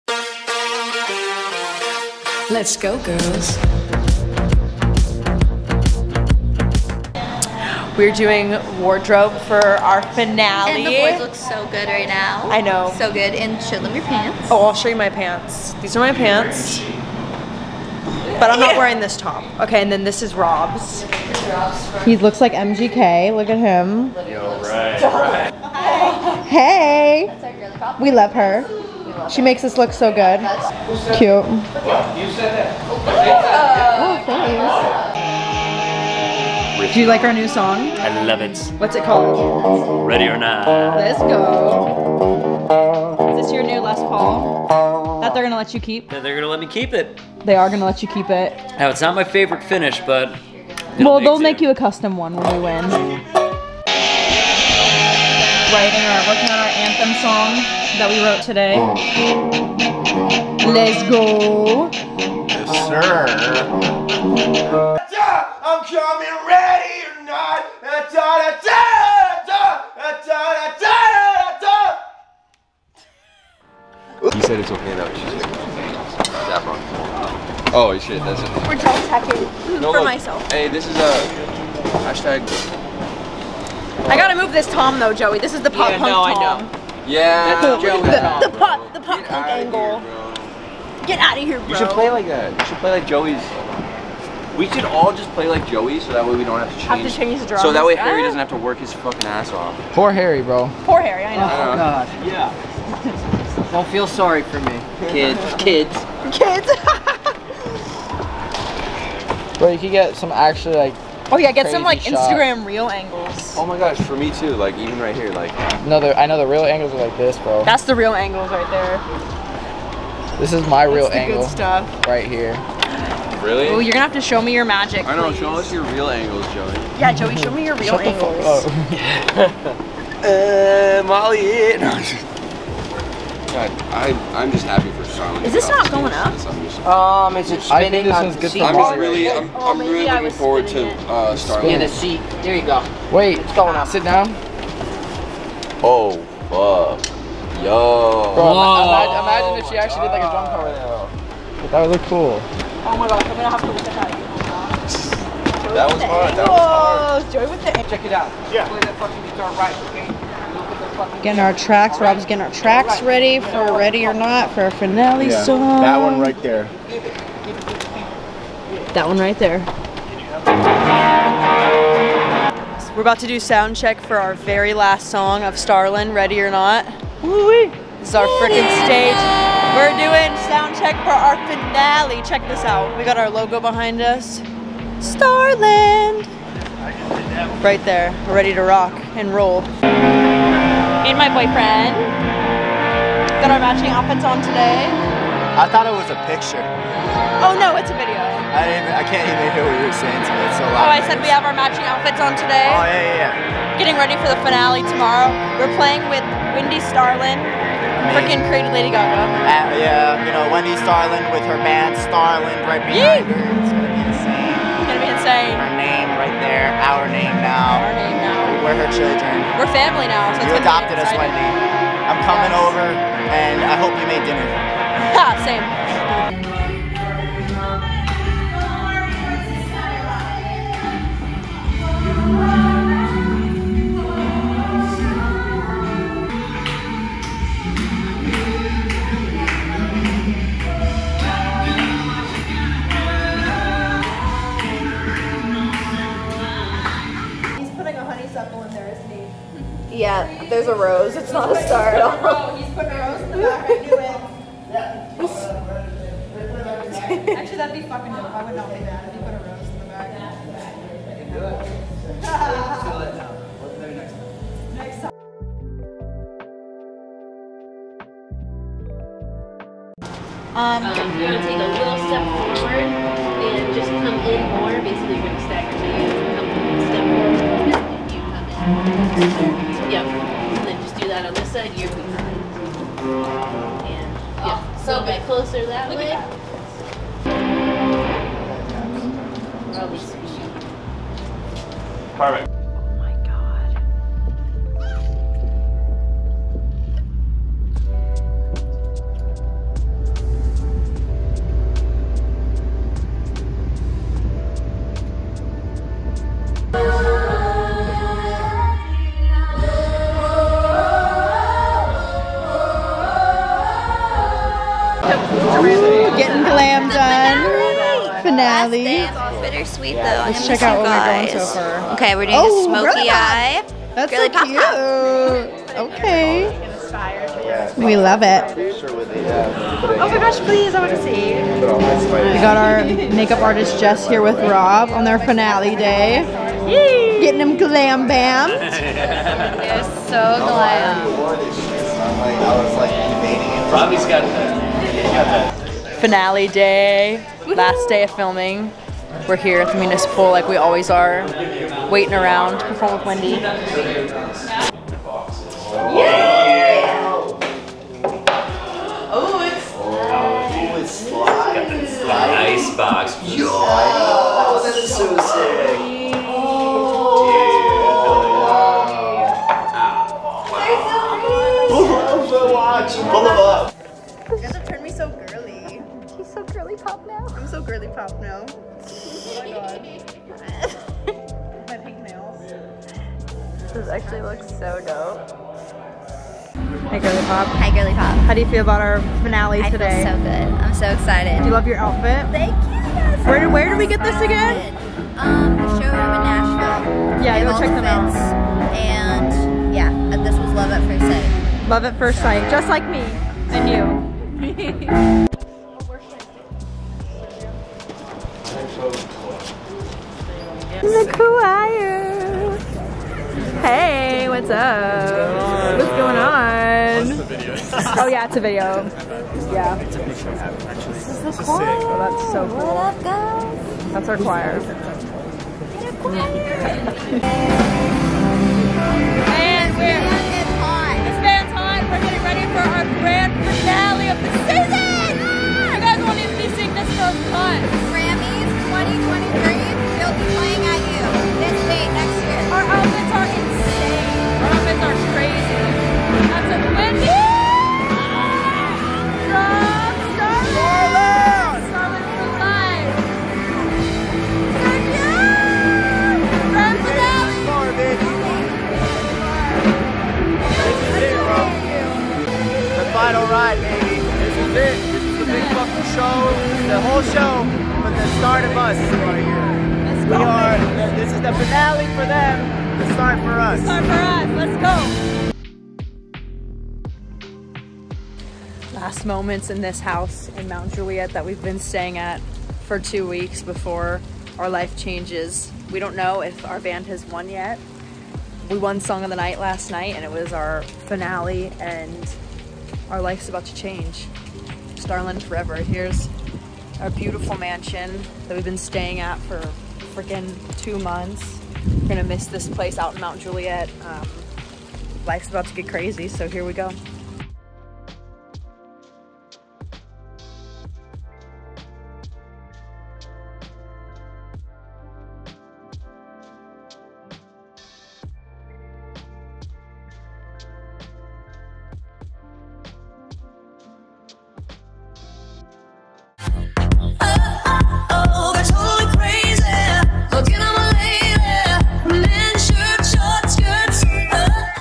Banded Finale VLOG | Behind the scenes of filming a TV show
Every wonder what it's like filming a TV show!? Go behind the scenes with me and see what it was like filming the first season of a BRAND NEW TV SHOW "Banded The Musician Competition".

During my 2 months living in a house with 25 other people I documented the whole experience. This is my last vlog video of the days and day of the finale.
WATCH BANDED ON AXS TV @banded/
Follow my Band Starland on Instagram @dband/

Endorsements: Paiste Cymbals, Vater Drumsticks, Cympad, Outlaw Drums, Big Fat Snare Drum, & RTOM